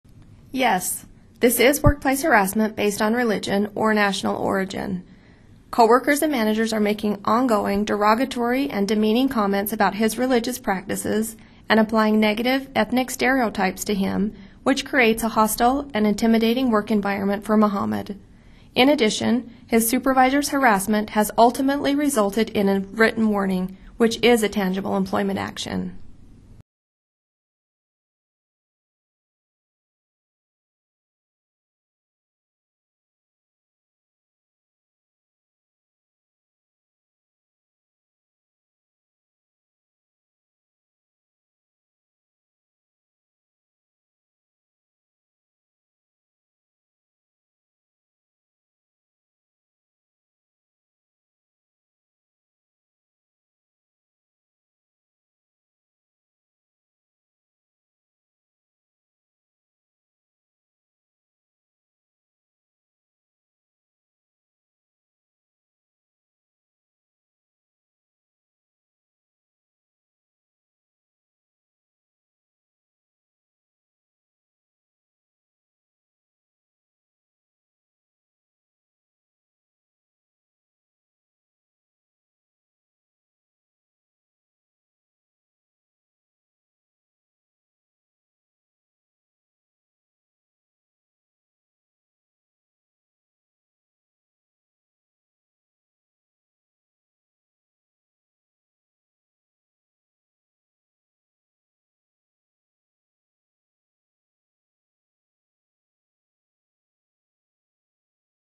Workplace Harassment Prevention Training (IYTV-16)
PA 6a 0:28
PA 6b 1:08

All employees of the State of Utah are required to take Workplace Harassment Prevention Training at least once every two years. This training will take approximately 20-25 minutes to complete.